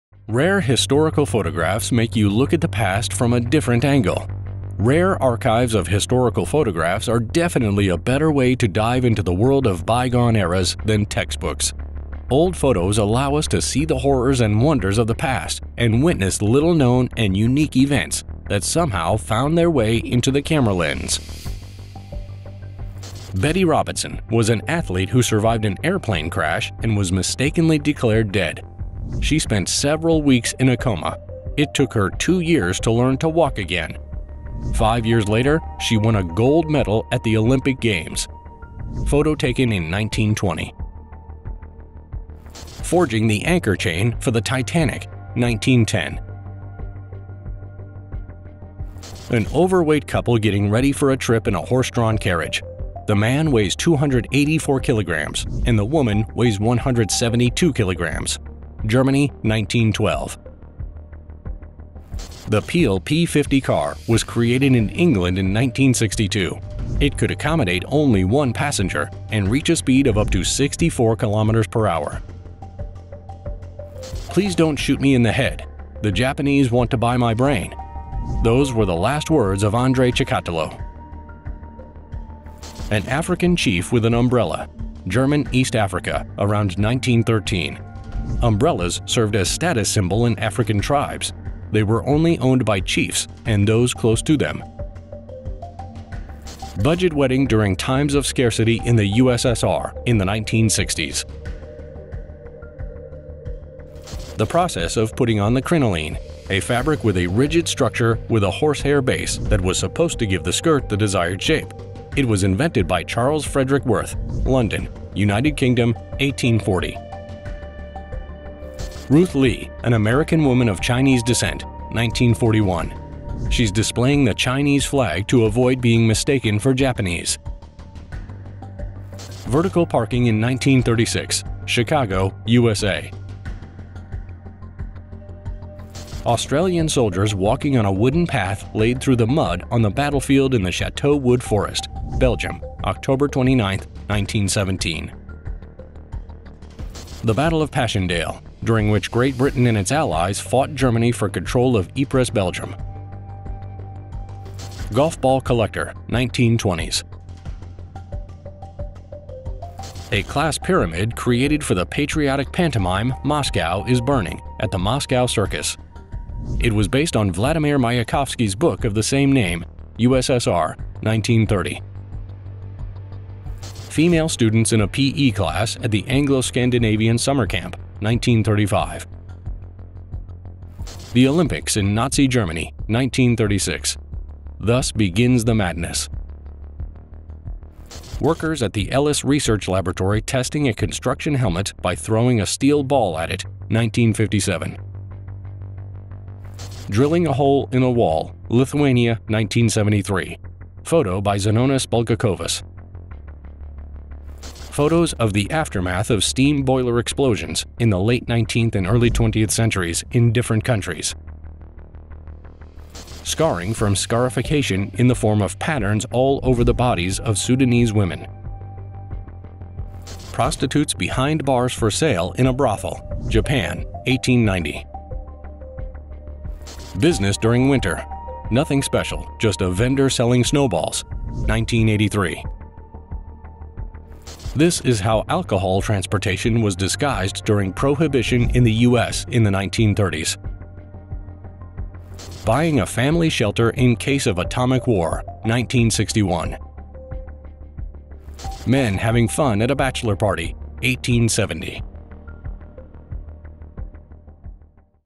These Old Photographs Will Change Your Perception of the Past! (Part 12)
Rare historical photographs make you look at the past from a different angle. Rare archives of historical photographs are definitely a better way to dive into the world of bygone eras than textbooks. Old photos allow us to see the horrors and wonders of the past and witness little-known and unique events that somehow found their way into the camera lens.